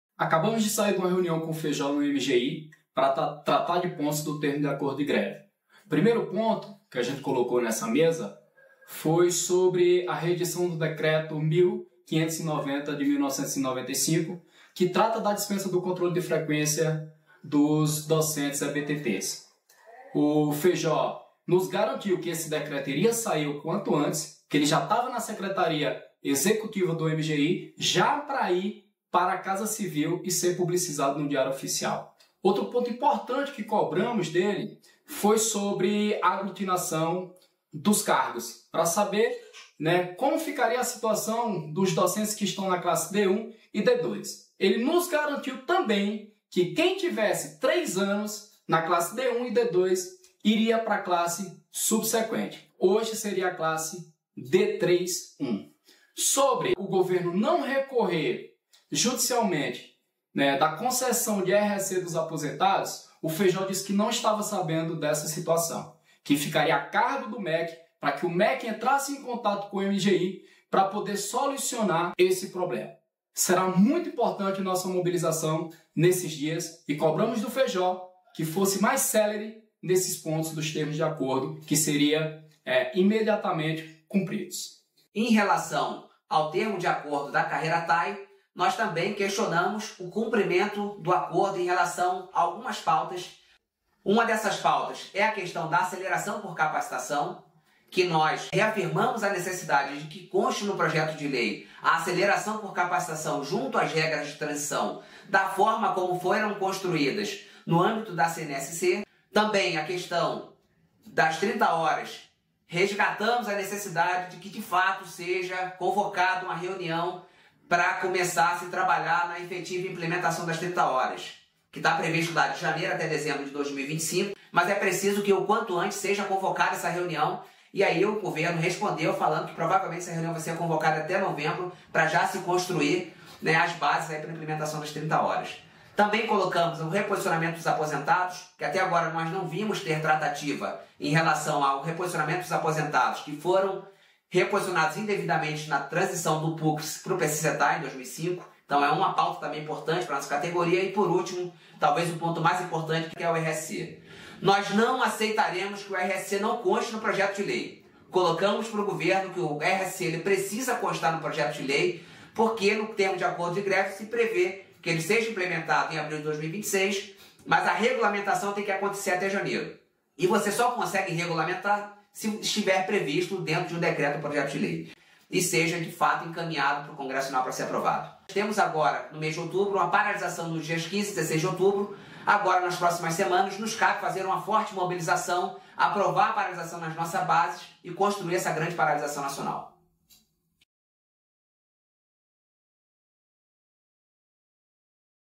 Informe de reunião com MGI em 02/10/2024
O SINASEFE se reuniu com o Ministério da Gestão e da Inovação em Serviços Públicos (MGI) na tarde desta quarta-feira (02/10). A pauta da reunião foi a cobrança do cumprimento dos acordos da Greve 2024.

O sindicato reafirmou a urgência do assunto, em especial diante da demora no cumprimento de itens assinalados como imediatos no termo docente e da possibilidade da exclusão de itens do projeto de lei relativo aos TAEs.

O SINASEFE foi representado na reunião pelo coordenador geral de plantão, Antonildo Pereira e pelo 2º tesoureiro e plantonista, Elton John.